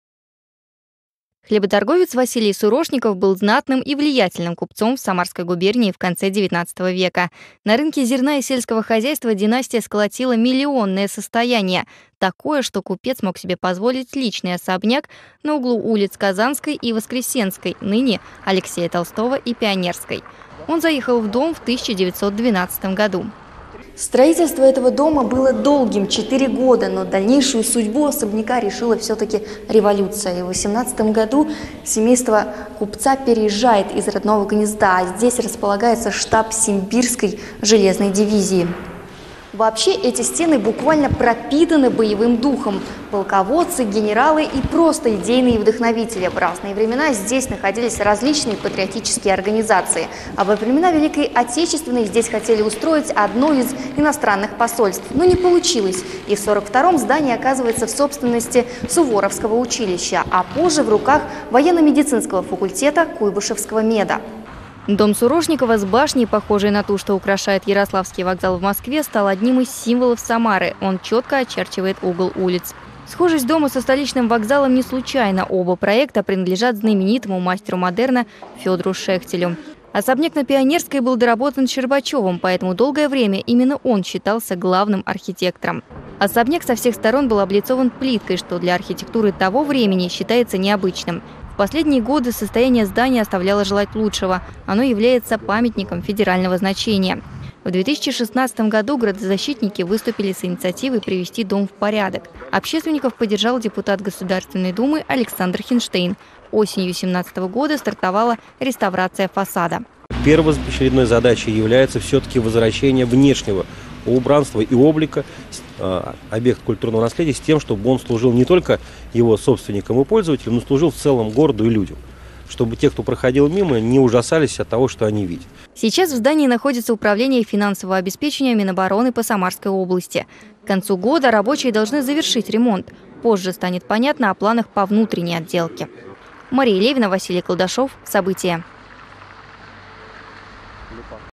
В Самаре заканчивается реставрация фасада особняка Сурошникова
Виртуозное исполнение проекта в графике, чувство такта и вкуса позволяют отнести этот дом к лучшим проектам Шехтеля. Так отзывались именитые критики о самарском особняке на Пионерской. В 1912-м его владельцем стал крупный хлеботорговец Василий Сурошников. Со времен революции стены этого дома обогатились историей. Из-за насыщенной судьбы здание пришло в негодность, решено было провести реставрацию фасада. За двери особняка одного из самых влиятельных купцов Российской империи заглянула Мария Левина.